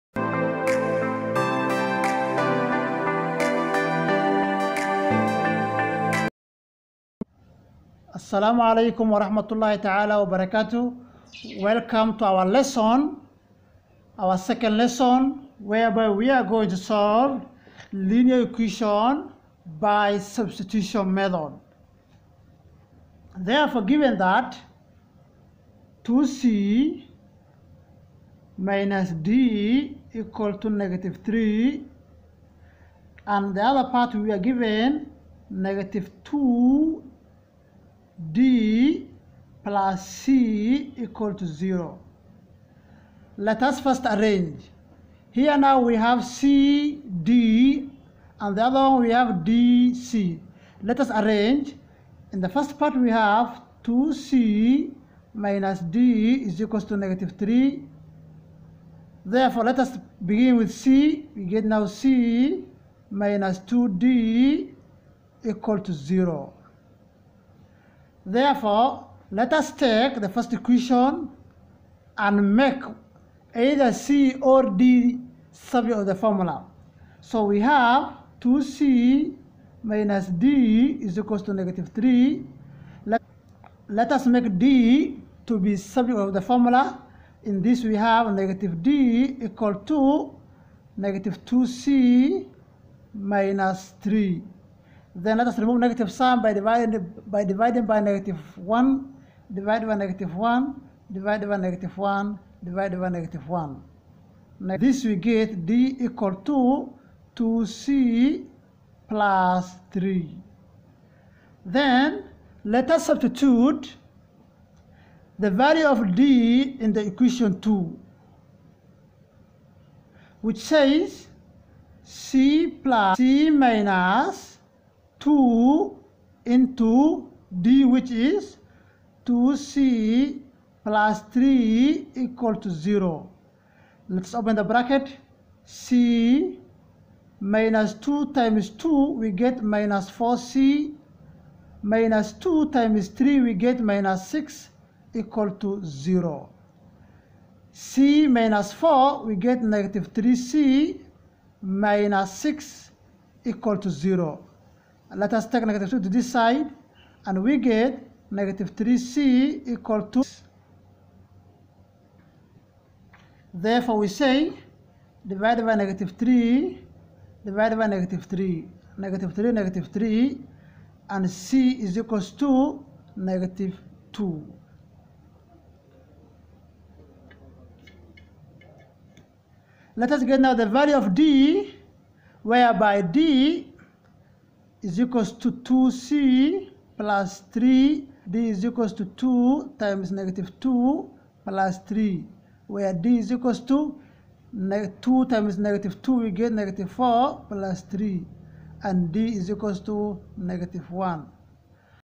LINEAR EQUATION:SOLVING SIMULTANEOUS EQUATION PART 2 (substitution method)
This video helps you understand how to solve  simultaneous equation using substitution method.